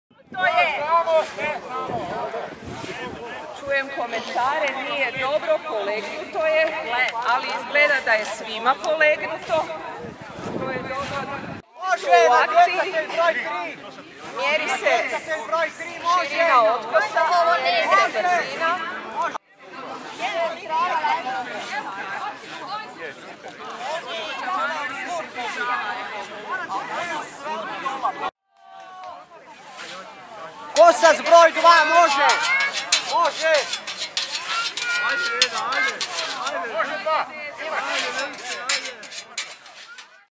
Koprivnica: Na Danima travnjaka održano natjecanje u ručnoj košnji na stari način
Damir Špehar/Pixsell